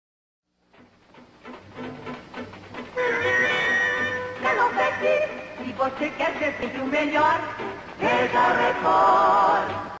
Rede Record - Vinheta de Interprogramas (1953)
A 1ª Vinheta da emissora, veículada de 1953 até 1970, a Record na época, foi a 2ª Emissora do Brasil á entrar no ar, e onde está até hoje de pé.

Créditos:
WSTV TELEVISÃO ___________________________________________________________________________